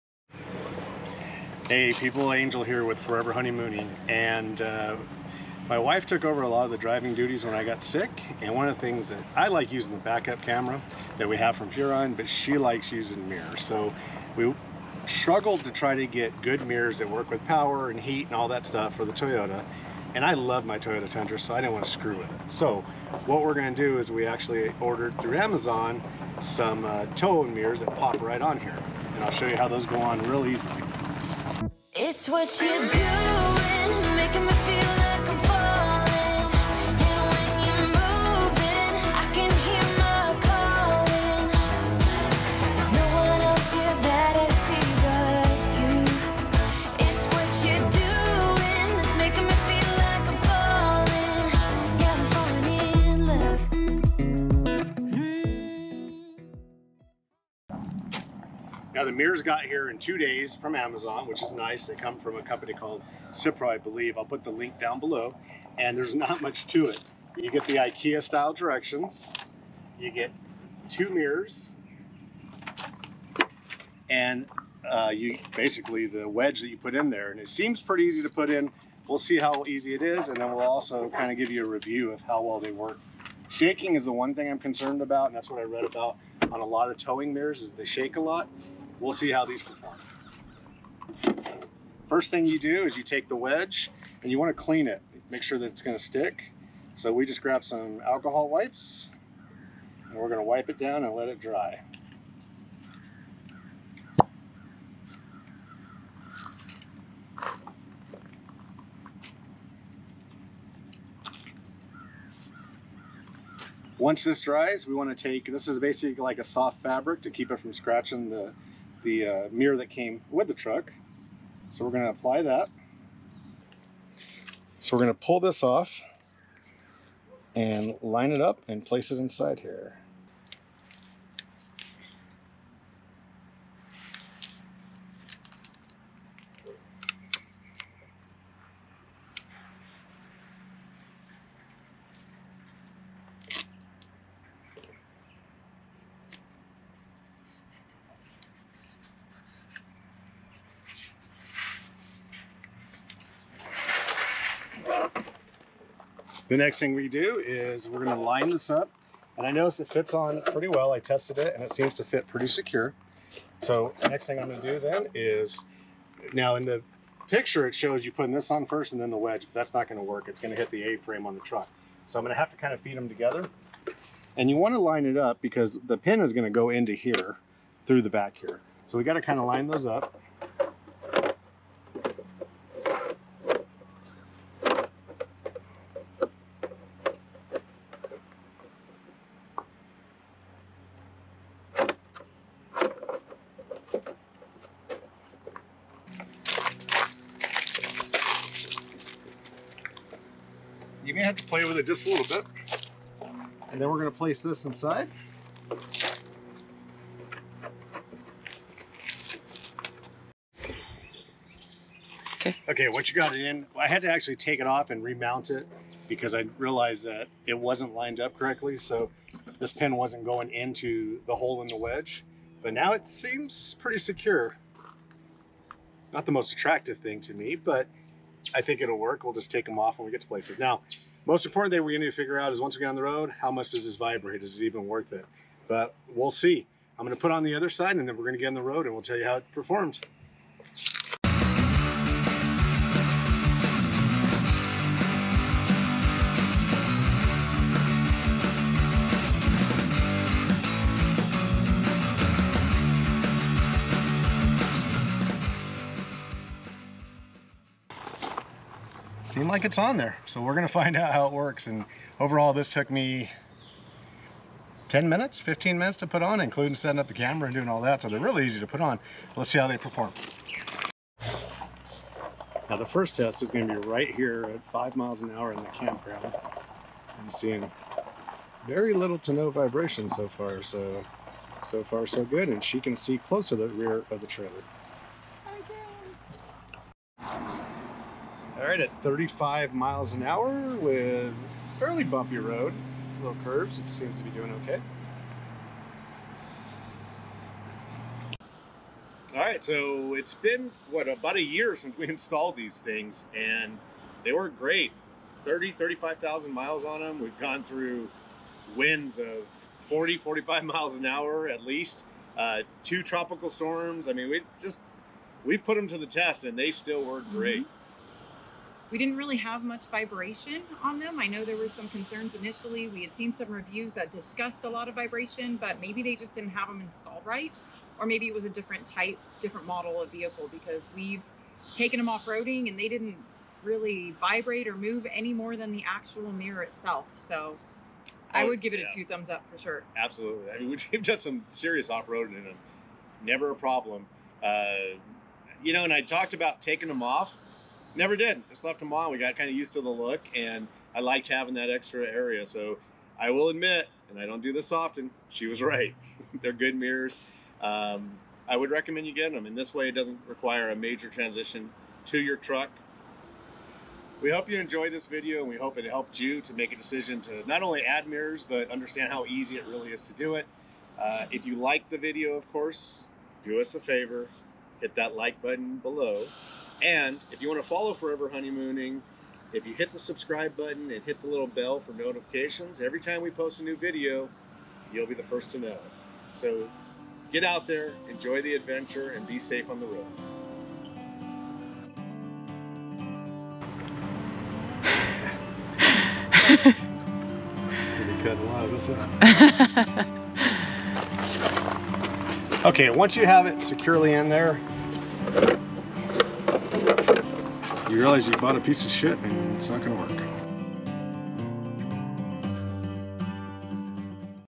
Easy temporary tow mirrors for any truck
These incredibly easy to install tow mirrors can be ordered for almost any truck. They are lightweight, easy to install, and worked great. We explain installation in just a few easy to follow steps that will get you on the road safely in just minutes. We also explain how well they performed in a variety of terrains and weather.

Mirrors for Toyota Tundra